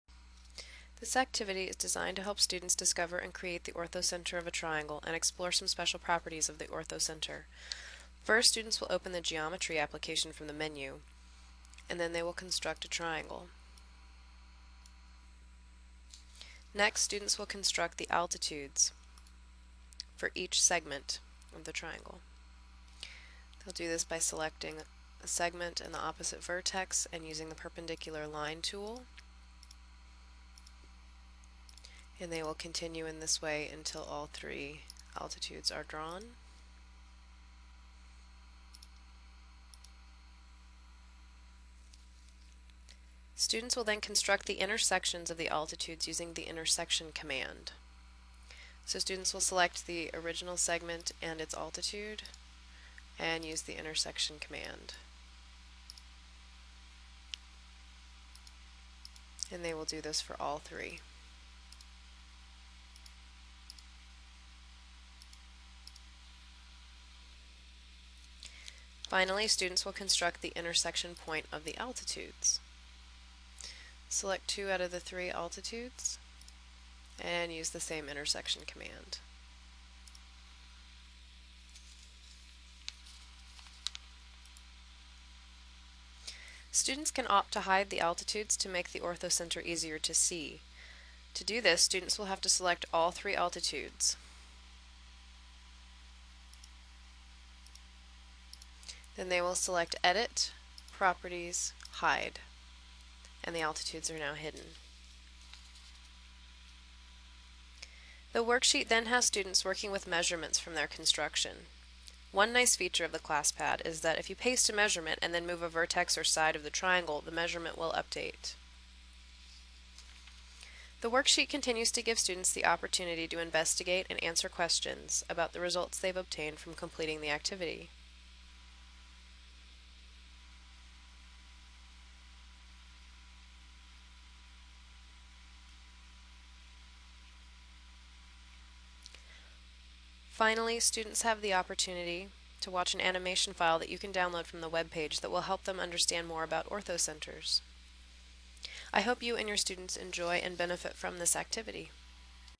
Orthocenter of a Triangle
In this activity students will construct the orthocenter of a triangle and learn about some of its properties. This activity was designed to be completed using the ClassPad 330 graphing calculator.